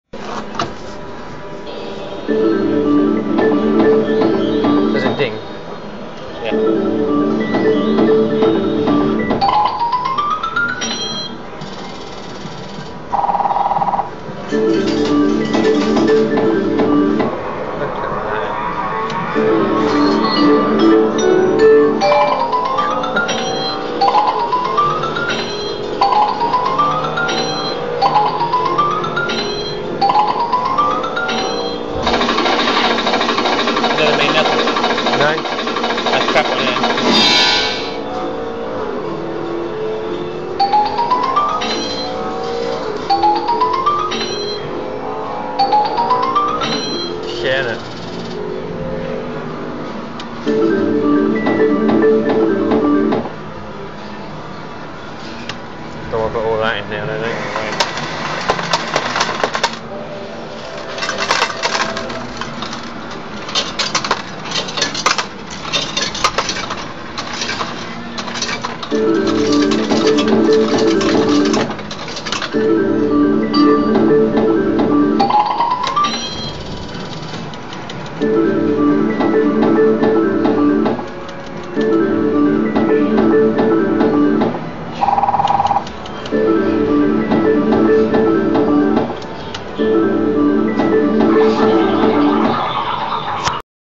Astra - Circus Time 3 Curtains CRAP!! B3
Here's clip of another option on the Party Slots machine. Got a feature aswell :-)